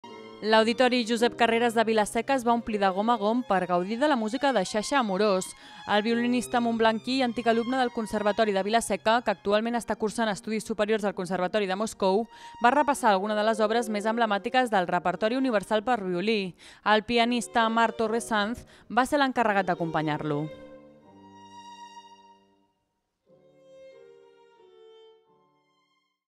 L'Auditori Josep Carreras s'omple per gaudir d'un jove talent violinista
Sasha Amorós va repassar algunes de les obres més emblemàtiques del repertori universal per a violí.